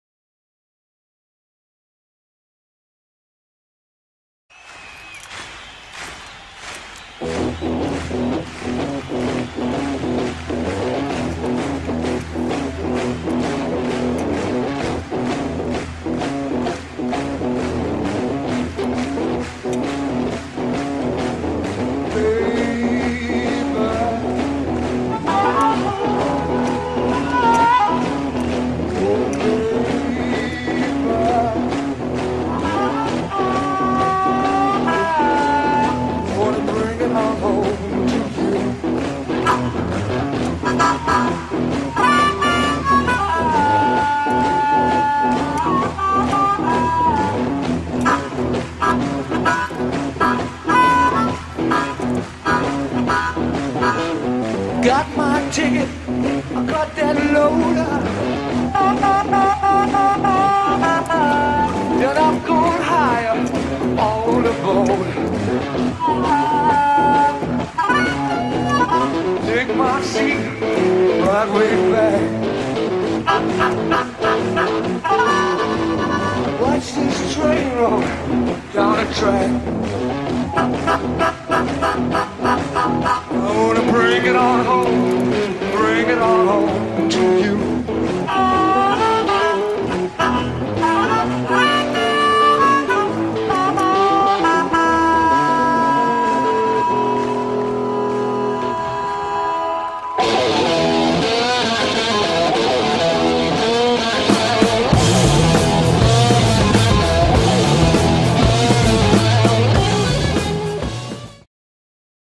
Led Zeppelin with vinyl
Led zeppelin characters produced using Adobe illustrator,Adobe Photoshop,Roland vinyl cutter ,Roland Cut Choice software
Apple imac computer using  iphoto,itunes ,imovie, and recorded on an iphone .